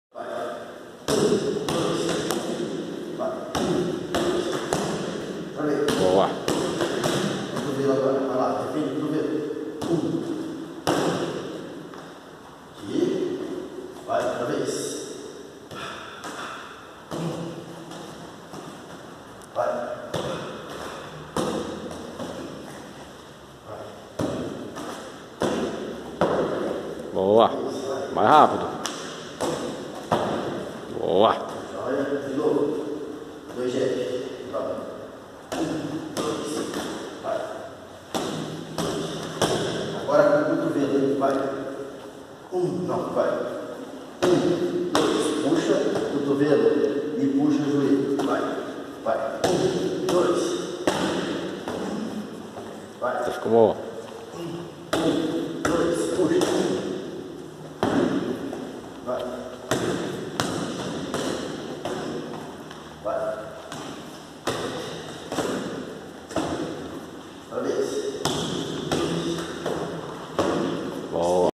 M.A.P. Sistema de Combate video 1
Treino de kickboxing e Muay Thai misto com Bloqueios e cotoveladas de Pangamut.Tecnica fora de regras para adaptacao pra defesa Pessoal etc...